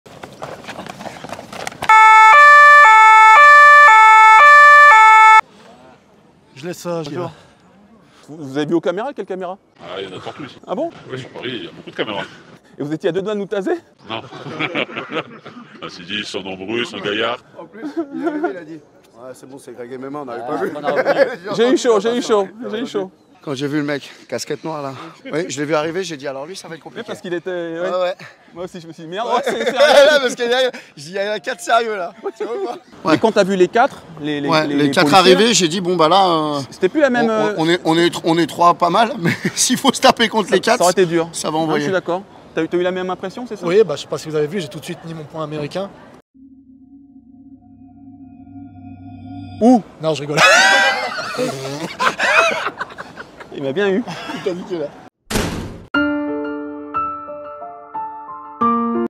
GregMMA interpellé par la police ! 😱
GregMMA retrouve Gaetan Le Bris et  son élève Steve Pasche pour des tests de rue. Ils ne sont pas au bout de leurs surprises...